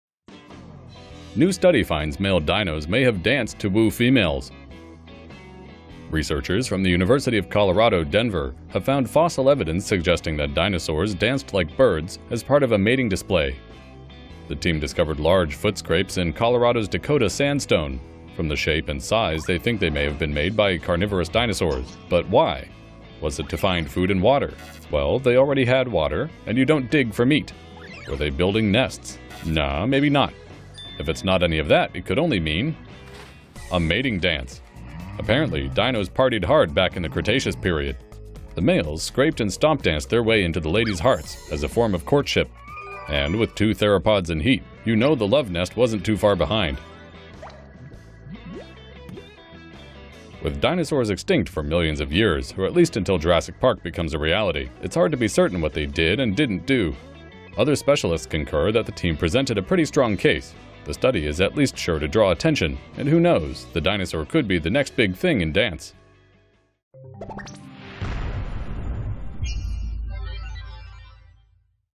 Disco dinos: Colorado researchers find carnivorous dinosaurs likely danced to woo females - TomoNews
DENVER — Researchers from the University of Colorado Denver have found fossil evidence suggesting that dinosaurs danced like birds as part of a mating display.

According to a study published in Scientific Reports, the team, led by geology professor Martin Lockley, discovered large foot scrapes up to two meters in diameter in Colorado's Dakota Sandstone. From the shape and size of the scrapes, the researchers think they may have been made by theropods, the carnivorous dinosaur family that counts the Tyrannosaurus rex as one of its members.

The question the team tried to answer is why the dinosaurs were digging. With water plentiful in the area, it surely wasn't for that. Nor was it for food, since the theropods ate meat, not plants. It did not seem connected to nest-building, either. What the researchers finally zeroed in on is the similarity of the scrapes to the nest scrape display, a mating ritual usually seen in modern birds.

The team hypothesized that like their descendants, the dinosaurs may have scraped and stomped on the ground in a form of mating dance to attract a female partner with which to later breed. The idea seems plausible considering that dinosaurs are believed to be modern birds' ancient ancestors.

But with dinosaurs having been extinct for millions of years, it's hard to be certain about what they did and didn't do. Other specialists, though, concur that the team presented a pretty strong case, and the study is at least sure to draw attention.